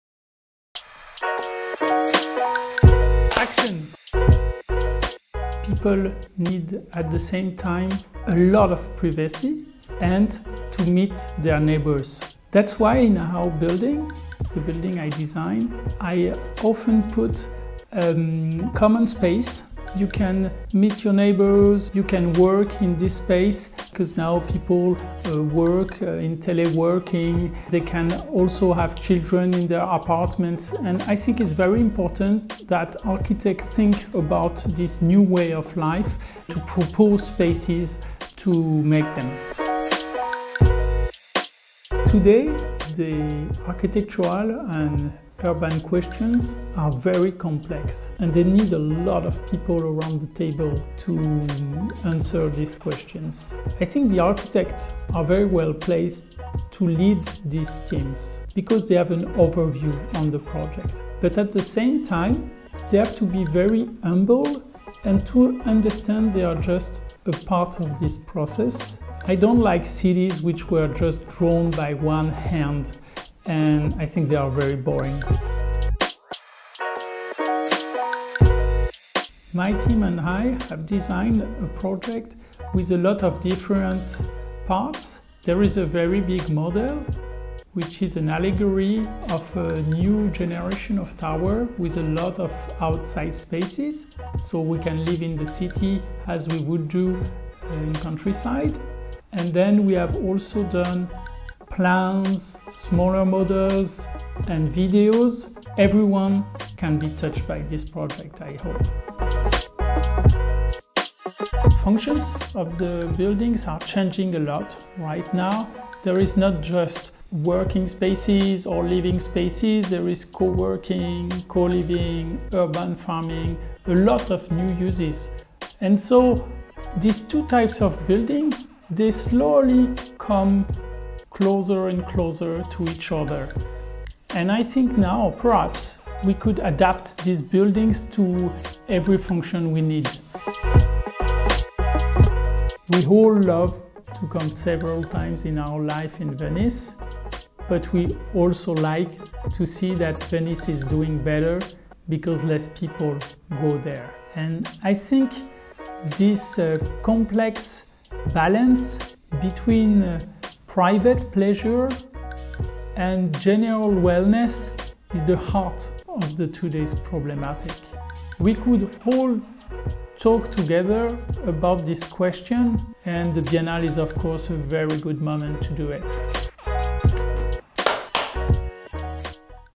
Biennale Architettura 2021 - Sneak Peek: nicolas laisné architectes
A sneak peek of the project by nicolas laisné architectes for the 17th International Architecture Exhibition "How will we live together?" (Venice, 22 May - 21 November 2021).